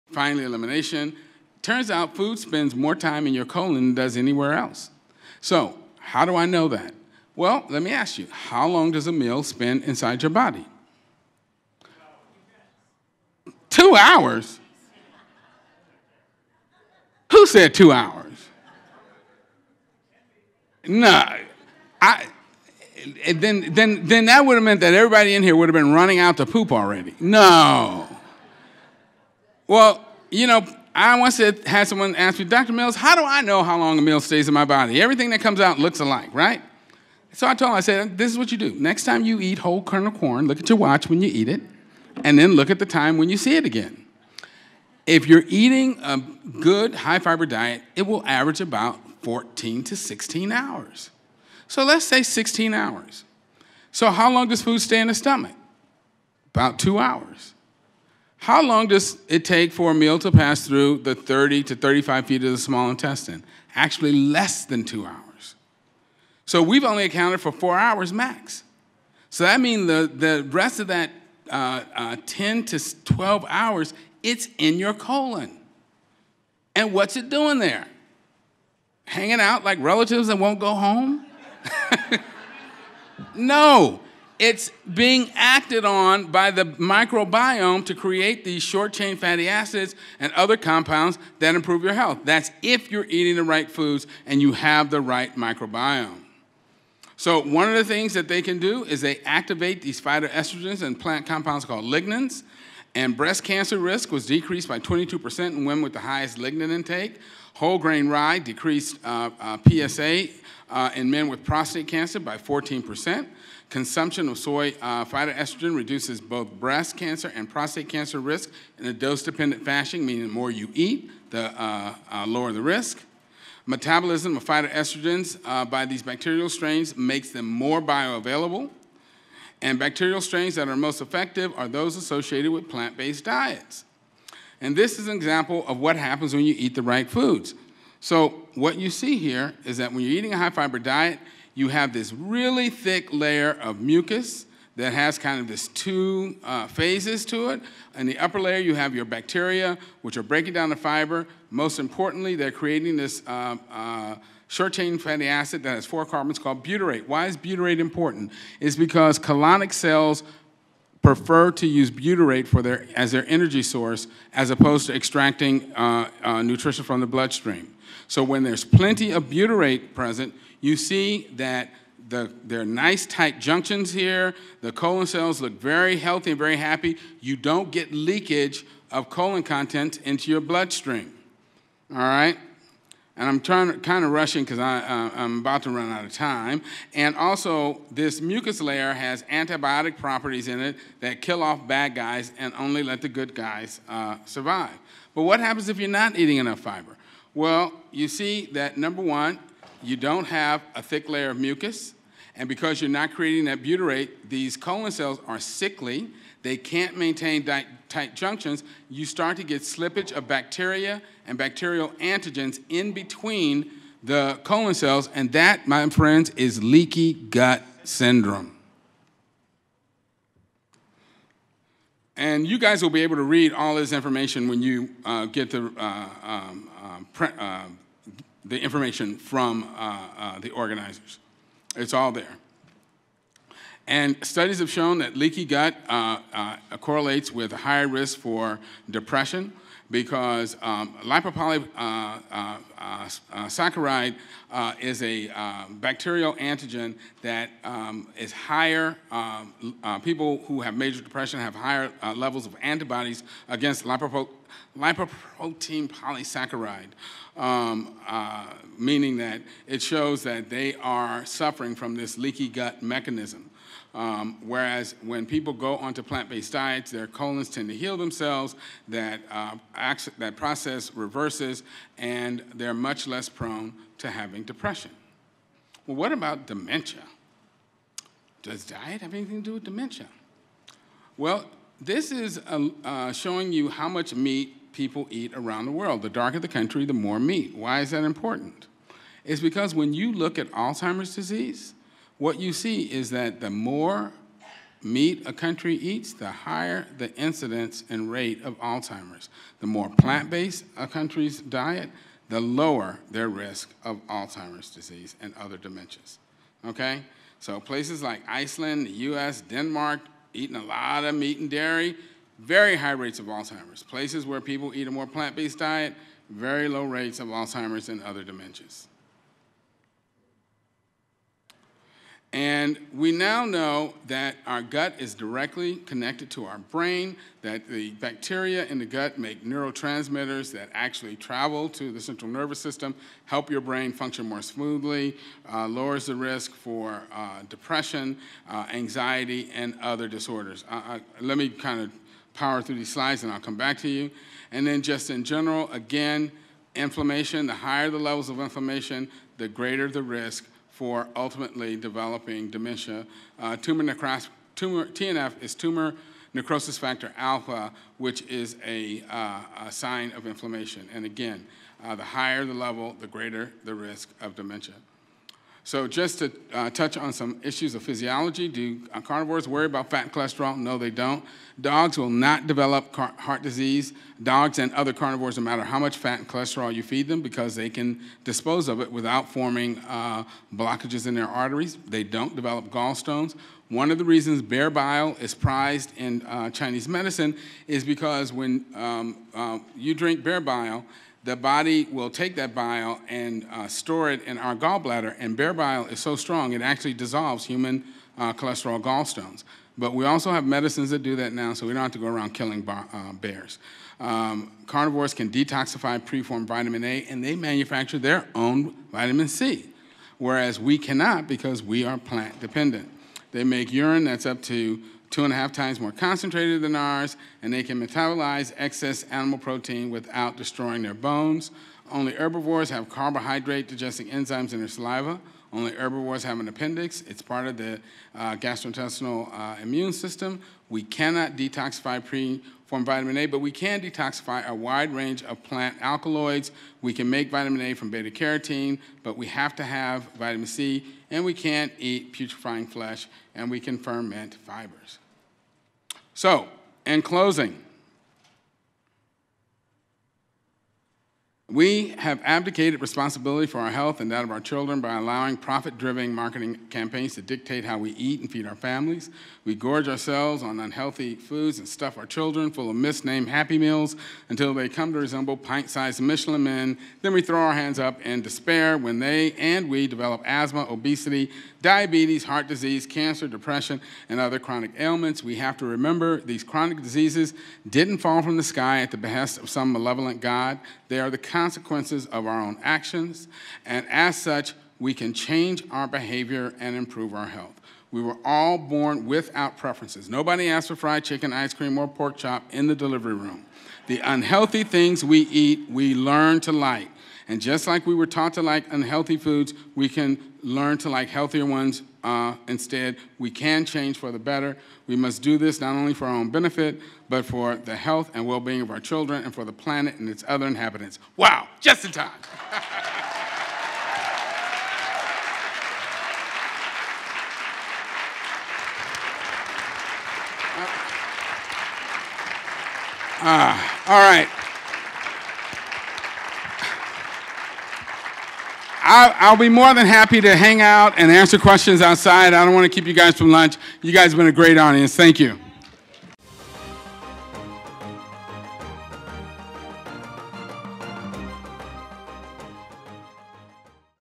The Impacts Of Plant Fiber On Chronic Disease - By Milton Mills
Milton Mills, MD practices urgent care medicine in the Washington DC area, and has served previously as Associate Director of Preventive Medicine and as a member of the National Advisory Board, for Physicians Committee for Responsible Medicine (PCRM). He has been a major contributor to position papers presented by PCRM to the United States Department of Agriculture regarding Dietary Guidelines for Americans, and has been the lead plaintiff in PCRM’s class action lawsuit that asks for warning labels on milk.

Dr. Mills earned his medical degree at Stanford University School of Medicine, and completed an Internal Medicine residency at Georgetown University Hospital. He has published several research journal articles dealing with racial bias in federal nutrition policy. He frequently donates his time via practicing at free medical clinics, and travels widely, speaking at hospitals, churches and community centers throughout the country. He was featured in the recent attention-getting film “What the Health,” and will also appear in the upcoming film “The Silent Vegan.”